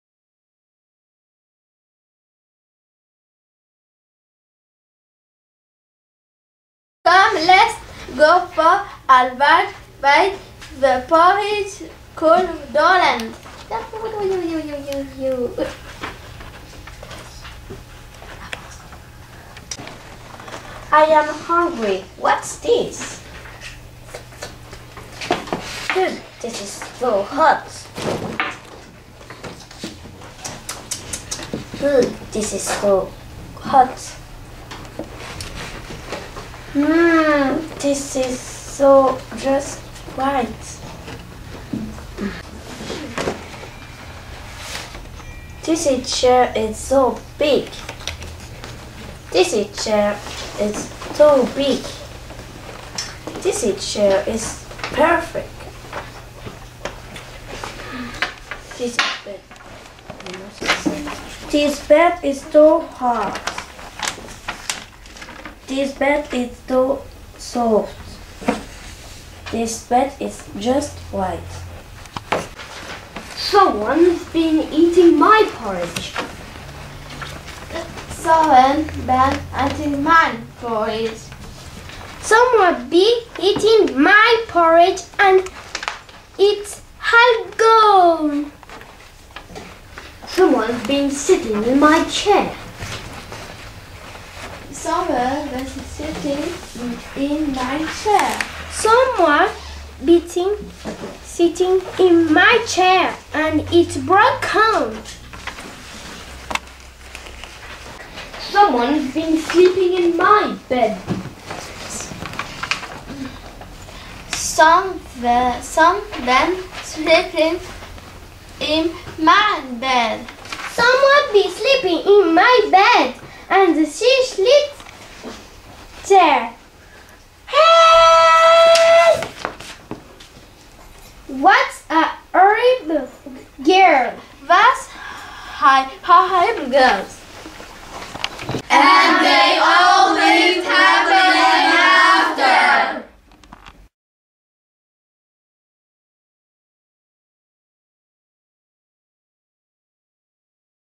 Goldilocks Group1
The story of Goldilocks retold by the primary of Lycée Topffer (March 2014).